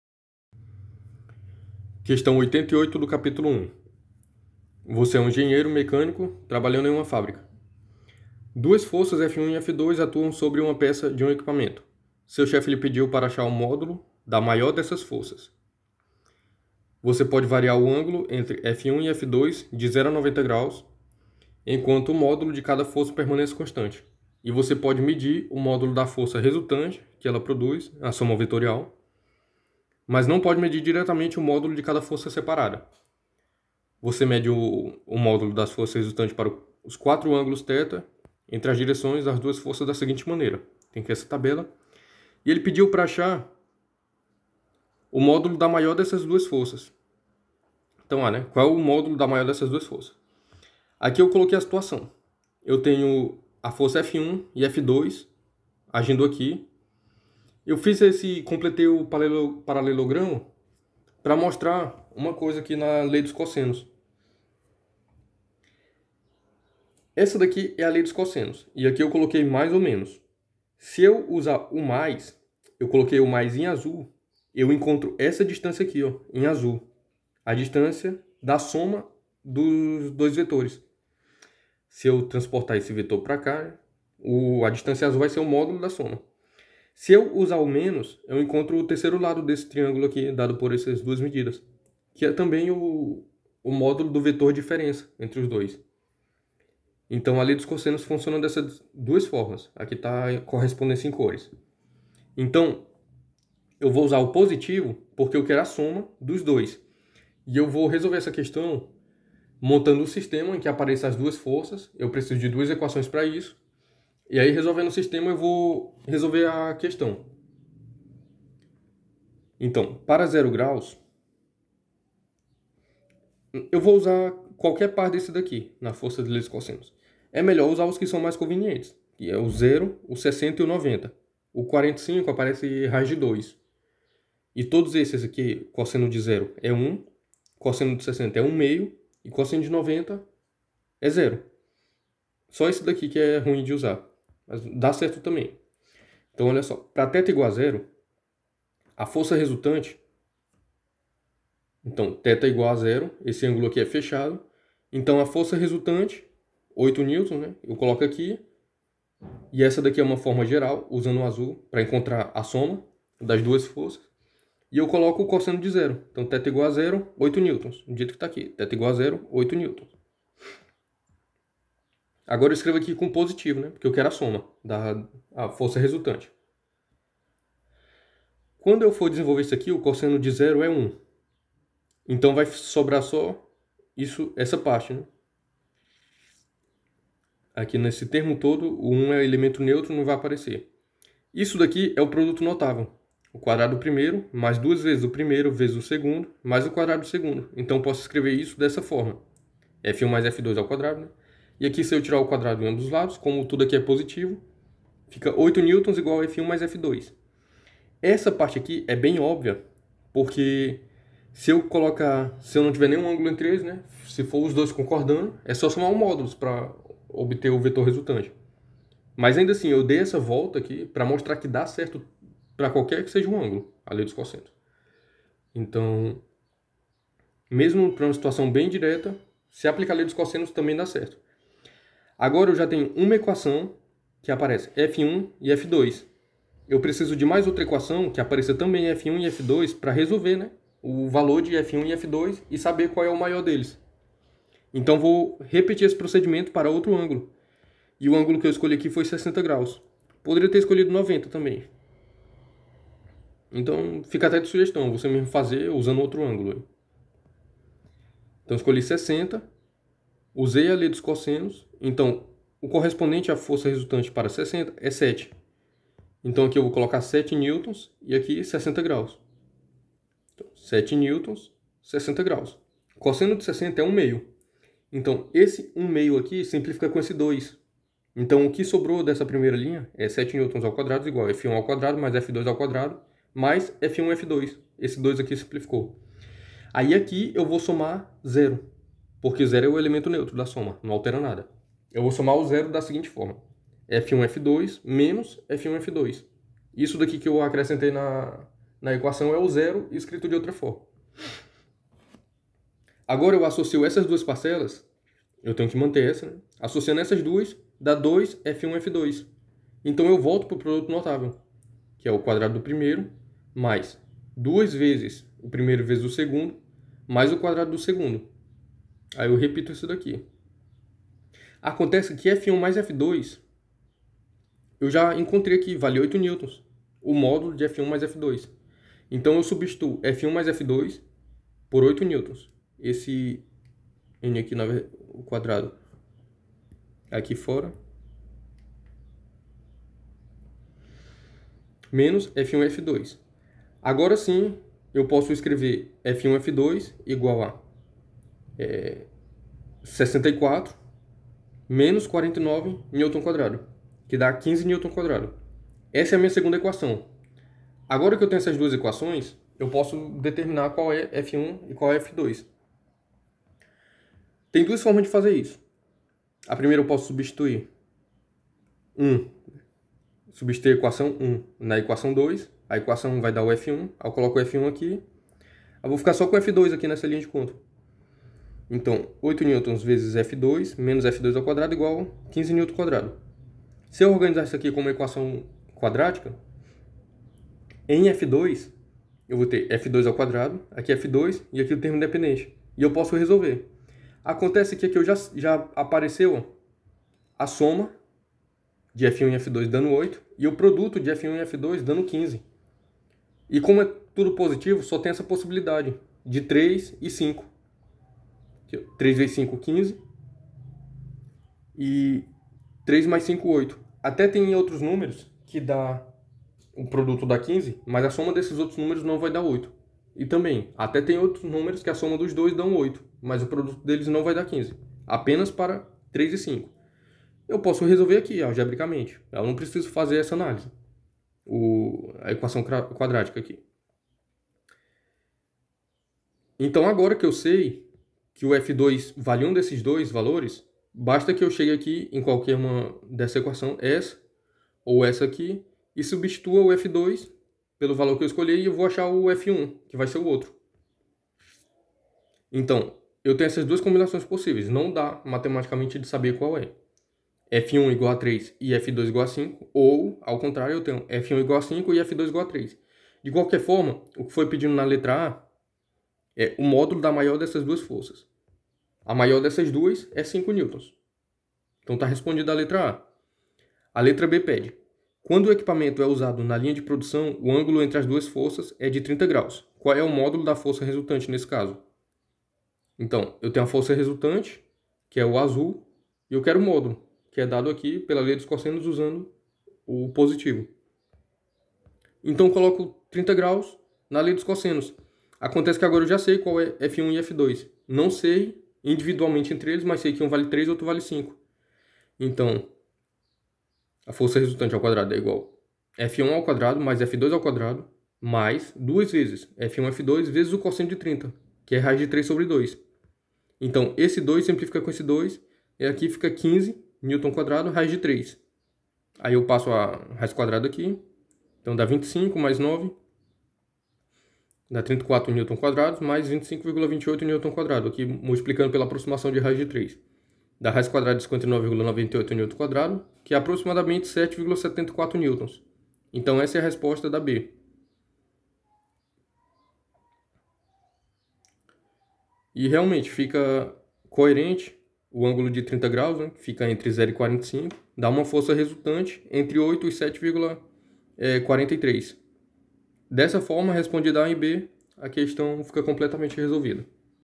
Questões Nível 3 - Física I - Mecânica - 14e | Cap 01 - Questão 1.88
Soluções das questões nível 3 do livro: Física I - Mecânica - 14e.

Questão: 1.88.

Pt: Você é um engenheiro mecânico trabalhando para uma fábrica. Duas forças, F1 e F2 atuam sobre a peça de um equipamento. Seu chefe lhe pediu para achar o módulo da maior dessas duas forças. Você pode variar o ângulo entre F1 e F2 de 0° a 90°, enquanto o módulo de cada força permanece constante. E você pode medir o módulo da força resultante que ela produz (sua soma vetorial), mas não pode medir diretamente o módulo de cada força separada. Você mede o módulo da força resultante para quatro ângulos theta entre as direções das duas forças da seguinte maneira:

(a) Qual é o módulo da maior dessas duas forças? (b) Quando o equipamento é usado na linha de produção, o ângulo entre as duas forças é 30,0°. Qual é o módulo da força resultante neste caso?

En: You are a mechanical engineer working for a manufacturing company. Two forces, F1 and F2, act on a component part of a piece of equipment. Your boss asked you to find the magnitude of the larger of these two forces. You can vary the angle between F1 and F2 from 0° to 90° while the magnitude of each force stays constant. And, you can measure the magnitude of the resultant force they produce (their vector sum), but you cannot directly measure the magnitude of each separate force. You measure the magnitude of the resultant force for four angles theta between the directions of the two forces as follows:

(a) What is the magnitude of the larger of the two forces? (b) When the equipment is used on the production line, the angle between the two forces is 30.0°. What is the magnitude of the resultant force in this case?